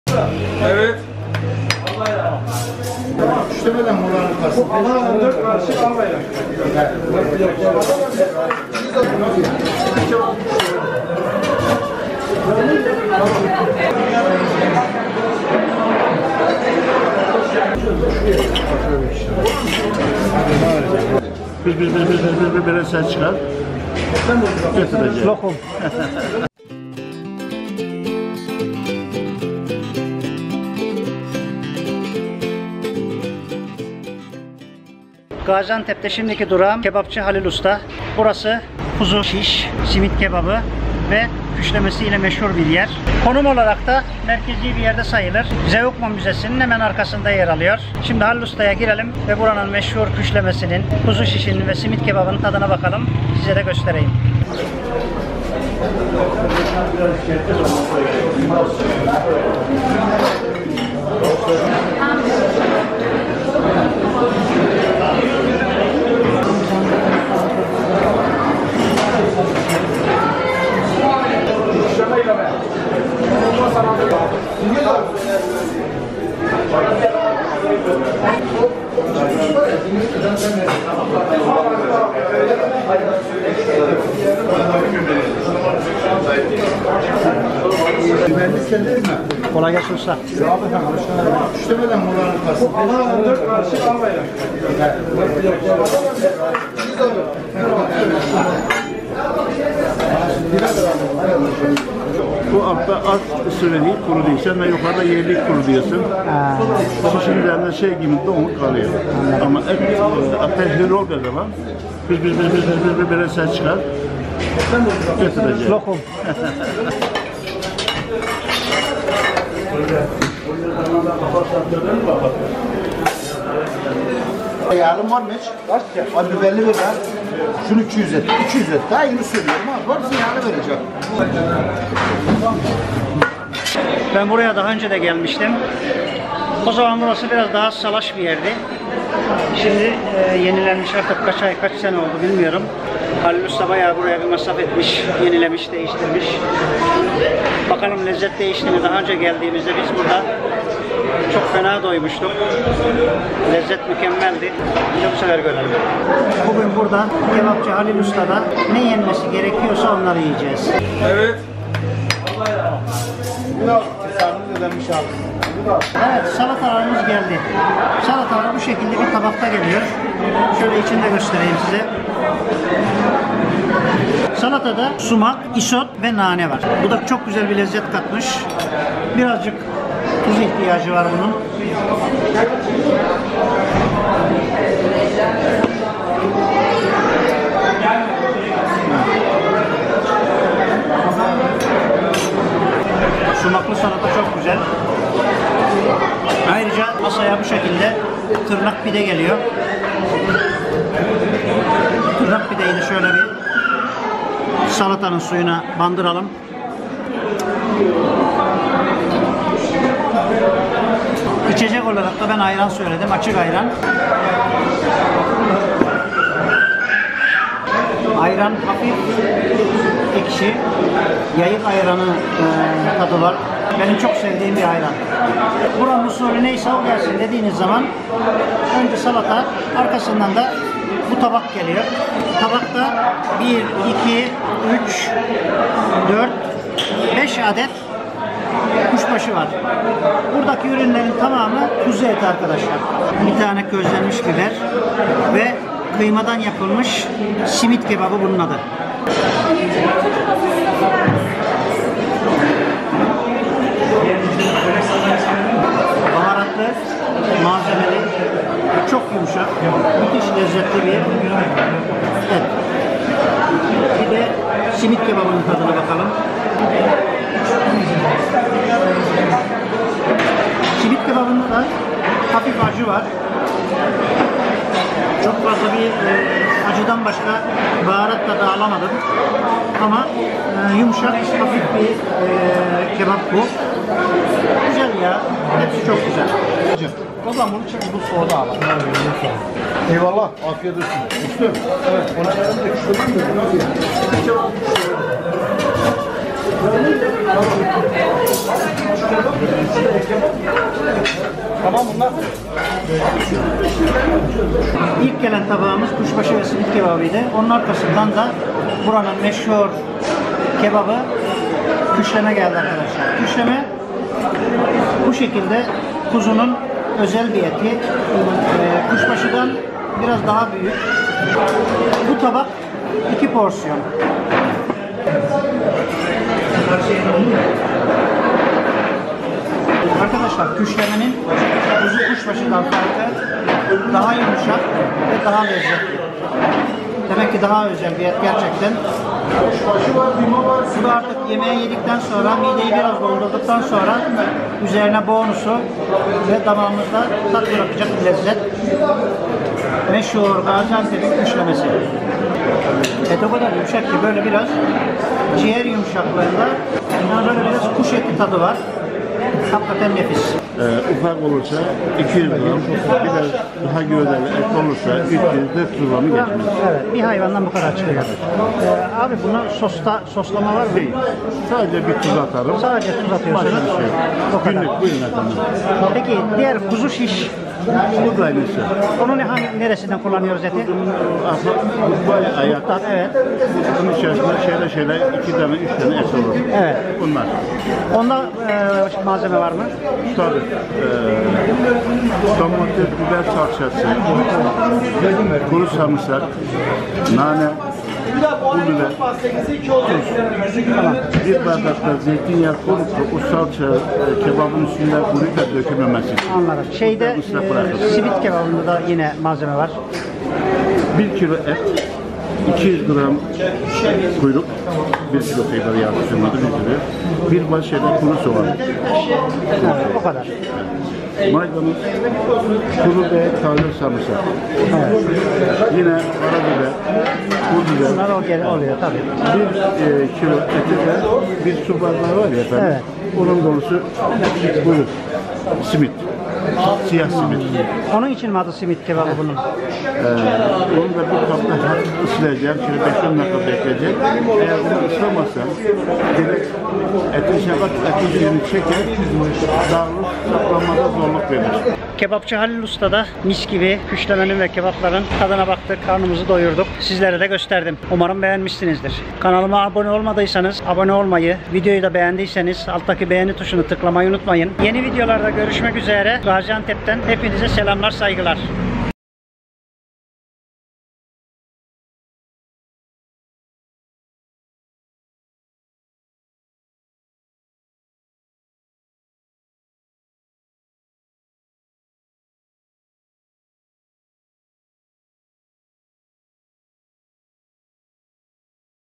KEBABABIN VE KÜŞLEMENİN ZİRVESİ | Kebapçı Halil Usta | GAZİANTEP
KEBABABIN VE KÜŞLEMENİN ZİRVESİ Kebapçı Halil Ustada damakları çatlattık. GAZİANTEP Kebap Turu Videosu...

Değerli dostlar Gaziantep gezimin 2. videosu ile karşınızdayım.
Bugün burada ünü tüm Türkiye'ye yayılmış Gaziantep'in meşhur küşlemecisi Kebapçı Halil Ustayı ziyaret edip nefis kebaplarının tadına bakıyoruz.
Hadi hem bana hem size kolay gelsin :)
Bu videoda yukarıda yazdıklarımı ve daha fazlasını bulabilirsiniz.